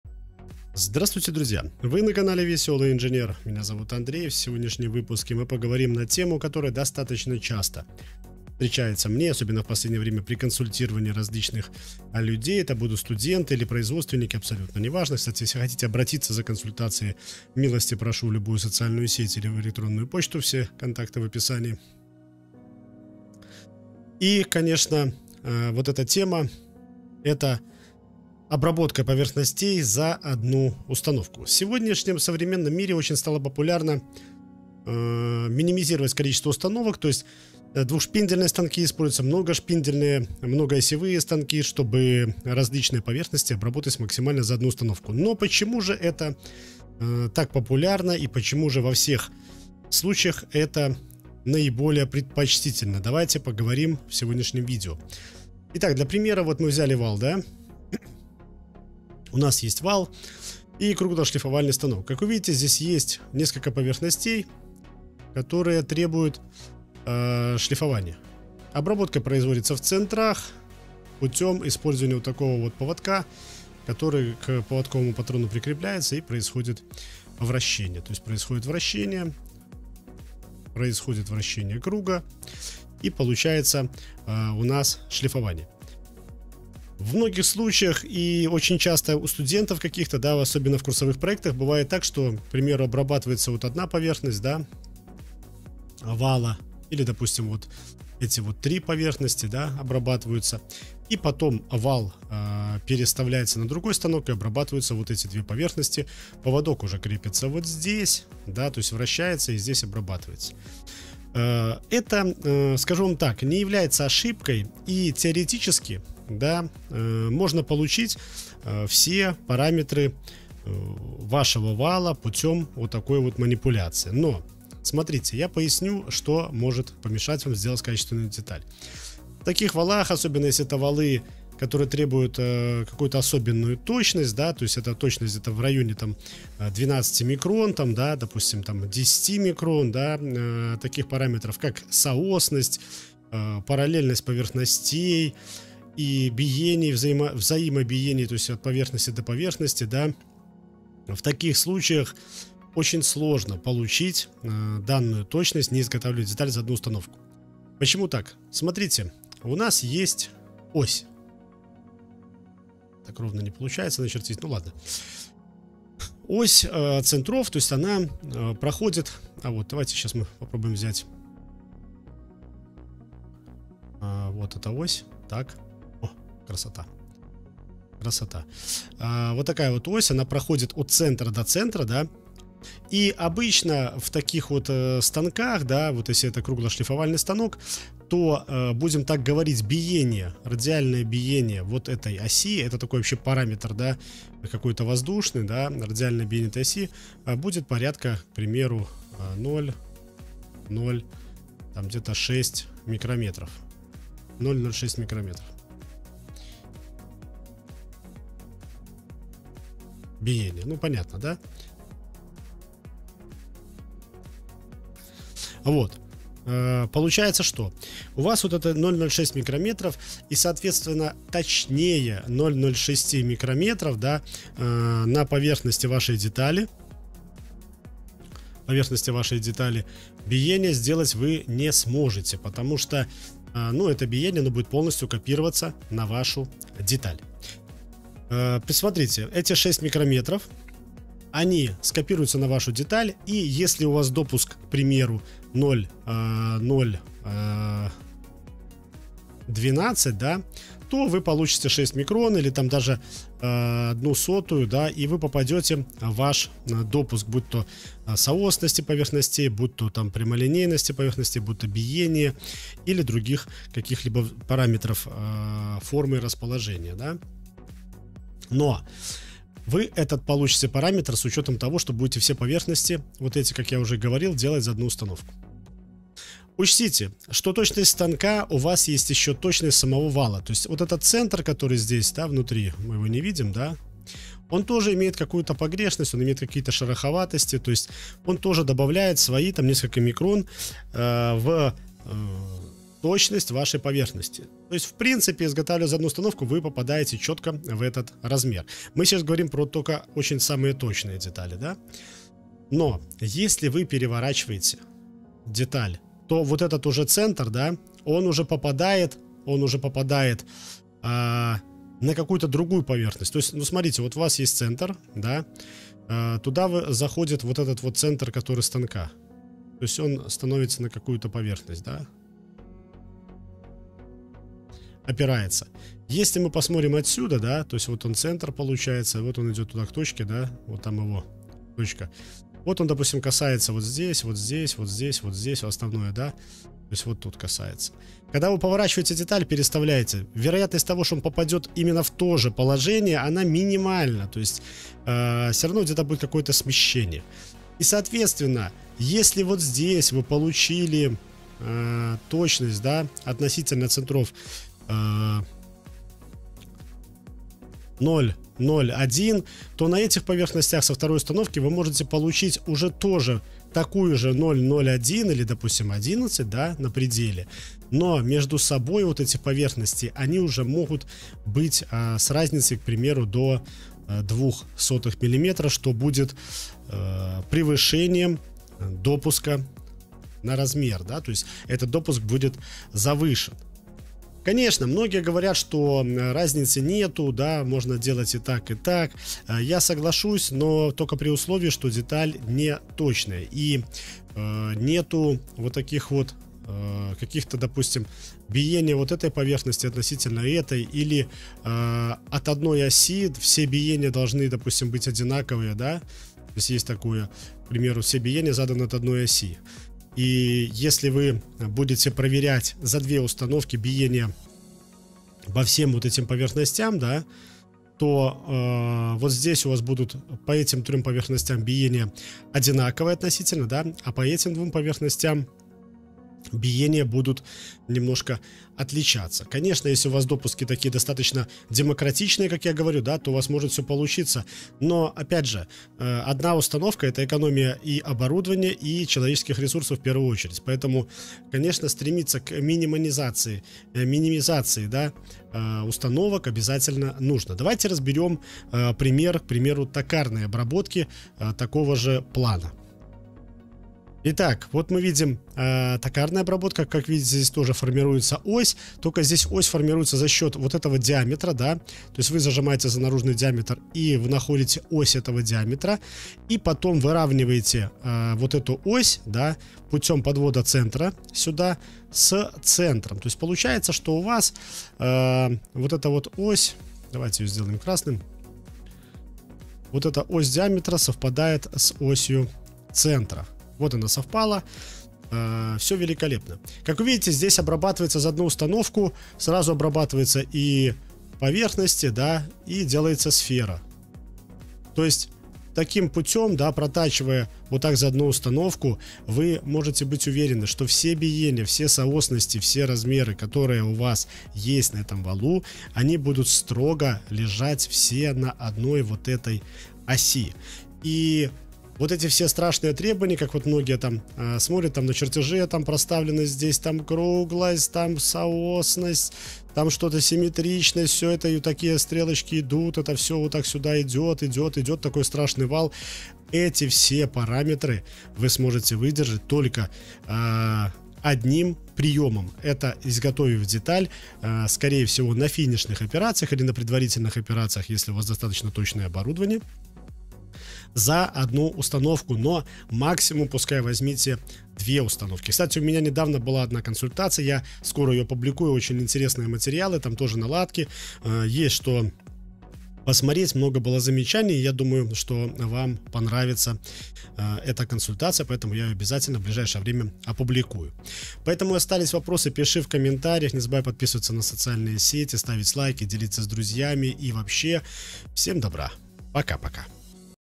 Обработка детали за одну установку! Как выдержать самые точные допуска формы и расположения?
В этом видео будет не просто токарная обработка, а объяснение принципа обработки детали за одну установку. Скажу вам честно, что обработка на токарном станке применяется почти на каждом предприятии машиностроения. Если ваши допуски кусаются и кажется, что выдержать их нет возможности. А если я вам скажу, что обычная токарная обработка деталей, позволит вам получить это все на обычном станке? Да, вы не ослышались, обычная токарная обработка резанием. Когда-то в техникуме я не подозревал, что это так просто, да и я сам не верил, что токарная работа может позволить изготовить такую точную деталь. У нас в курсовых проектах, для таких целей применялось шлифование. В старые времена,  шлифование металла на станке, было чуть ли не самая точная операция, когда необходима финишная обработка вала на станке с ЧПУ. В общем в этом видео техника и технология переплетаются в единое целое.
Это не просто видео – это наглядная лекция по машиностроению, раскрывающая техпроцесс изготовления детали от эскиза до результата. Узнайте технологию машиностроения на практике! И да, наш веселый инженер сделает погружение в мир техники и технологии не только познавательным, но и увлекательным!